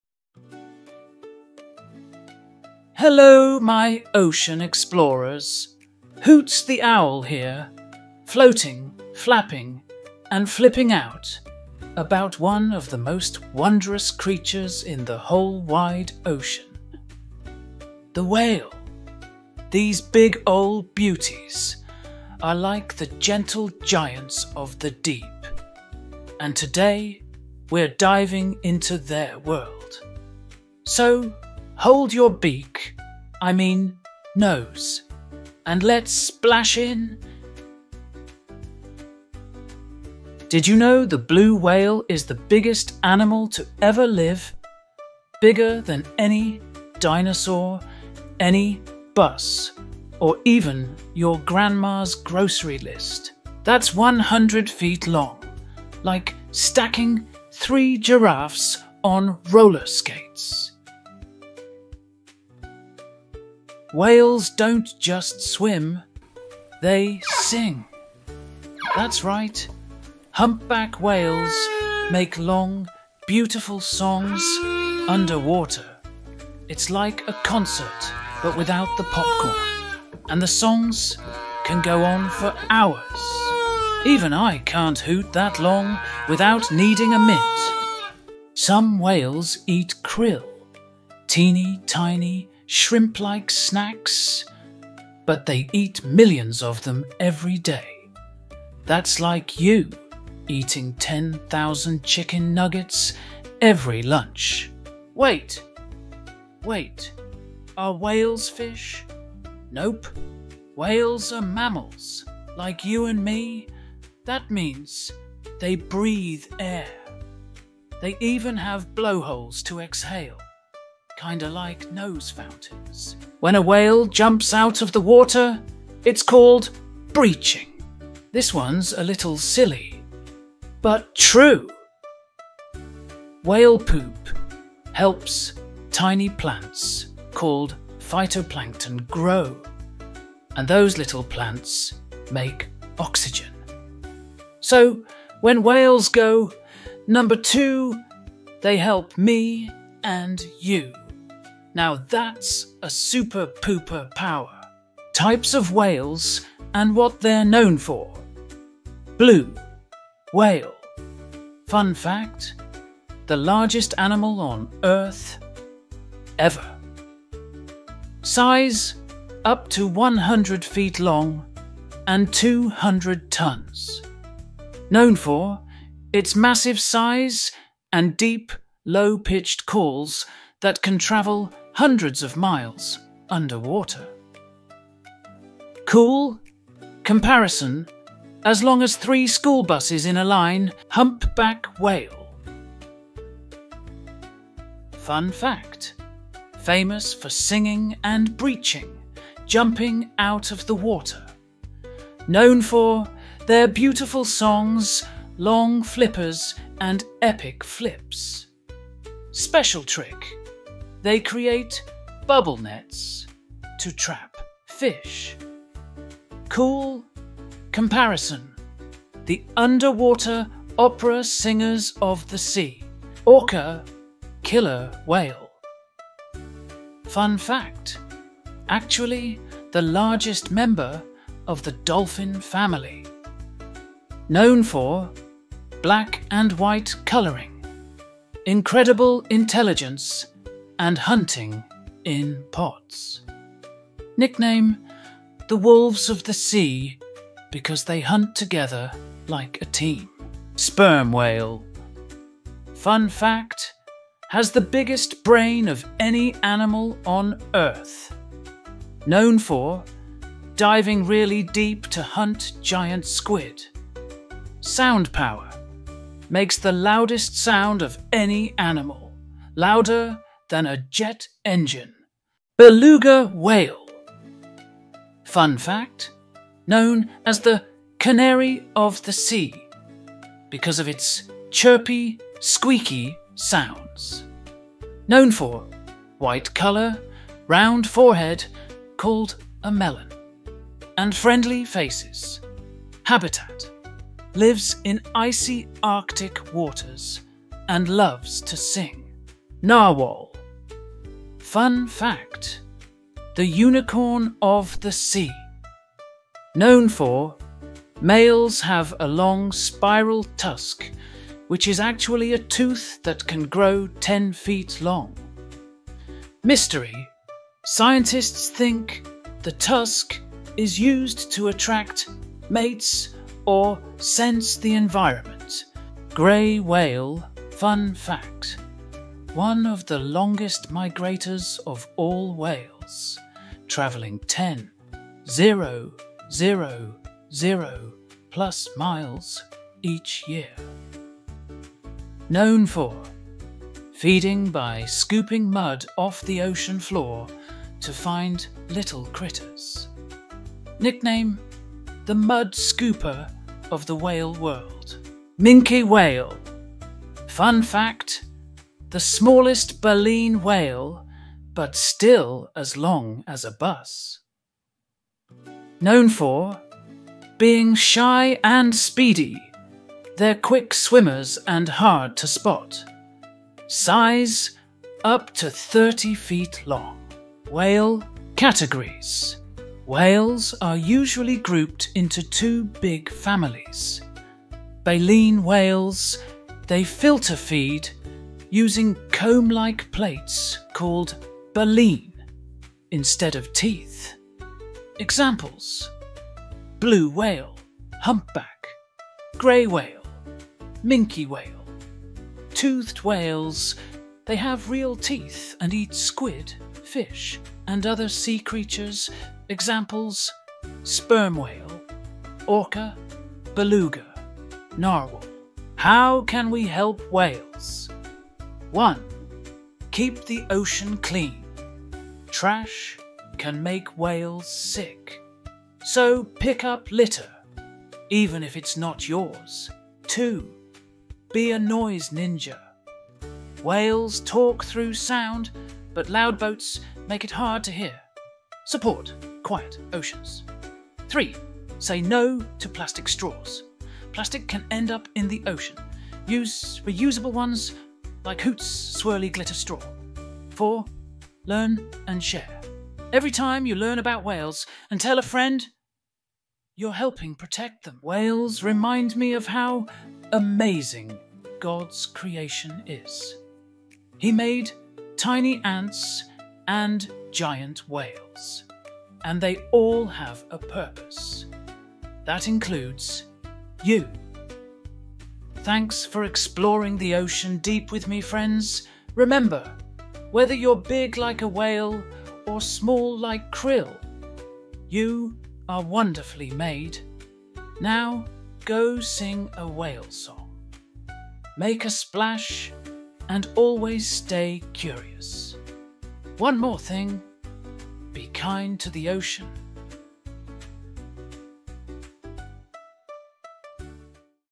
A whale Of A Time - Exploring the Amazing World of Whales
Get ready to splash into the BIGGEST adventure yet!
Join Hoots the Wise Owl as he dives into the deep blue sea to explore the amazing, silly, and surprising world of whales! 🐋

In this fun and educational video, kids will discover:
🌊 What makes whales so BIG and special
🎶 Which whale sings underwater songs
🎈 Why blowholes are like nose fountains
💨 How whale poop helps the planet (yes, really!)
🪄 AND the coolest whale tricks—from bubble nets to backflips!

PLUS: Learn how YOU can help protect these gentle ocean giants and become a Whale Hero! 🦸‍♂️💙

Whether you’re a curious kid, a homeschooler, or just love learning about animals, this video is packed with laughs, learning, and tons of whale-sized wonder!

📚 My Books